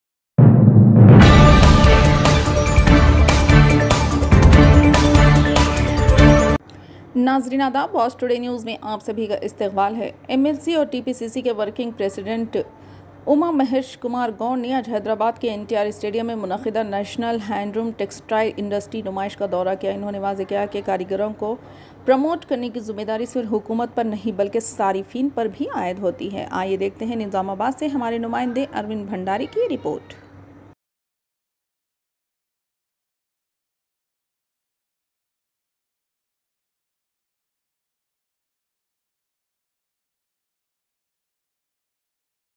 MLC & TPCC Working President Bomma Mahesh Kumar Goud visited the National Handloom Textile Industry
MLC & TPCC Working President Bomma Mahesh Kumar Goud visited the National Handloom Textile Industry Exhibition organized at NTR Stadium, Hyderabad today.

He made it clear that the responsibility of promoting artisans is not only on the government but on the consumers